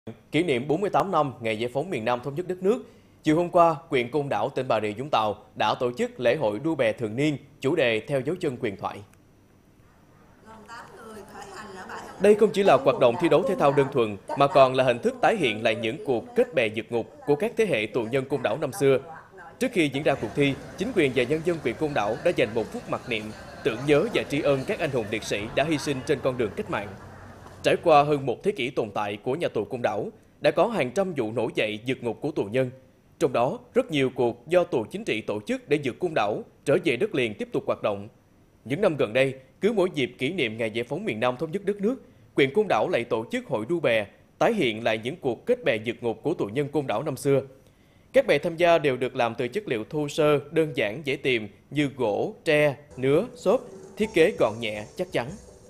Hội thi đua bè Côn Đảo “Theo dấu chân huyền thoại”
Kỷ niệm 48 năm ngày giải phóng Miền Nam, thống nhất đất nước, chiều hôm qua, huyện Côn Đảo, tỉnh Bà Rịa Vũng Tàu đã tổ chức Lễ Hội đua bè thường niên chủ đề “Theo dấu chân huyền thoại”. Đây không chỉ là hoạt động thi đấu thể thao đơn thuần, mà còn là hình thức tái hiện lại những cuộc “Kết bè vượt ngục” của các thế hệ tù nhân Côn Đảo năm xưa

⬇️⬇️⬇️ Tải THVL Audio để nghe đầy đủ